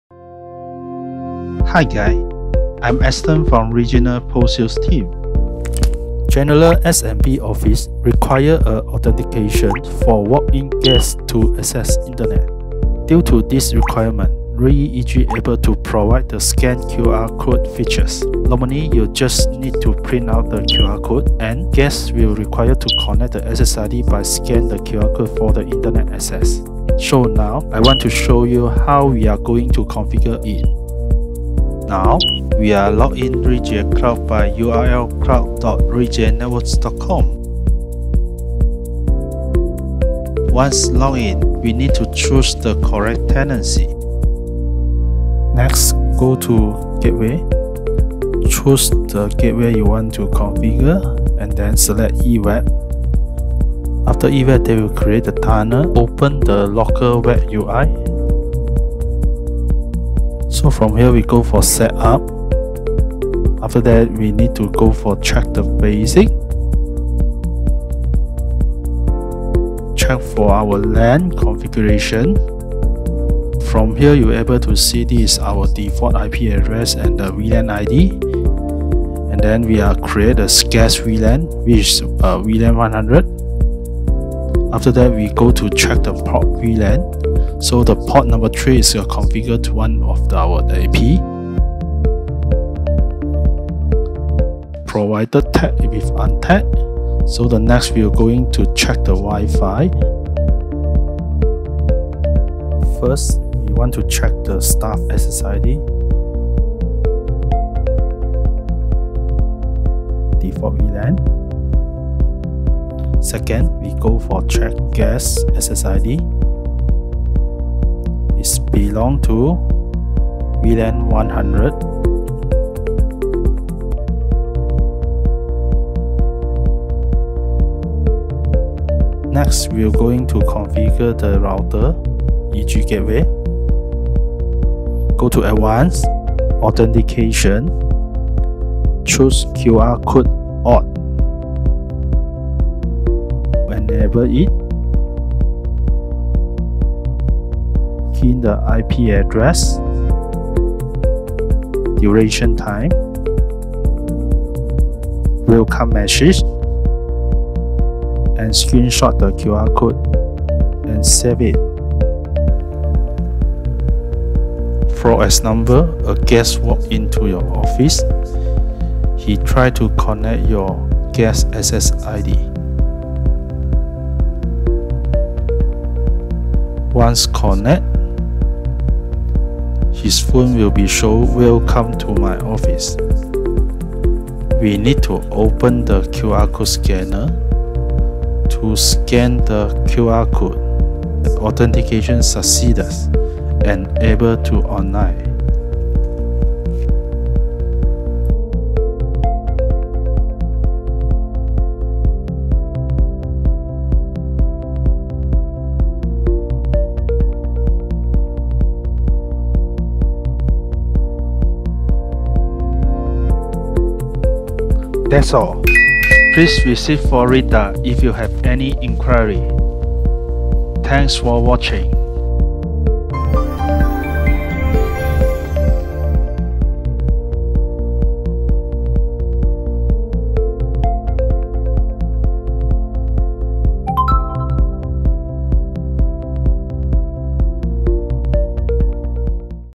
How to configure QRcode for guest internet access Reyee EG
Video Credit : Ruijie Networks